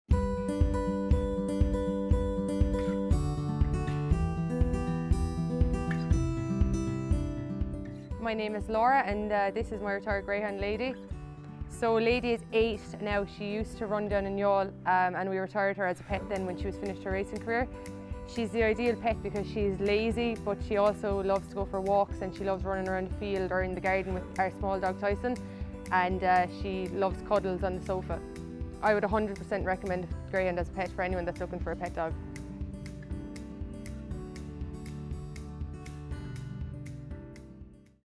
Tales from the Yellow Chair: Laura & Lady
"She's the ideal pet because she's lazy but she also loves to go for walks and she loves running around the field or in the garden with our small dog...she loves cuddles on the sofa."

Our Yellow Chair has been travelling around Ireland to capture the tales of best friends - retired Irish Greyhounds and their humans.

In this tale we travel to County Cork to meet  Laura and the beautiful Lady. Lady used to race in Youghal but now enjoys running around the garden instead. When she's not lazing on the couch for cuddles of course!

This clip was aired as part of the live greyhound racing TV broadcast of the BoyleSports Irish Greyhound Derby Final on Virgin Media 2 on Saturday 24th September.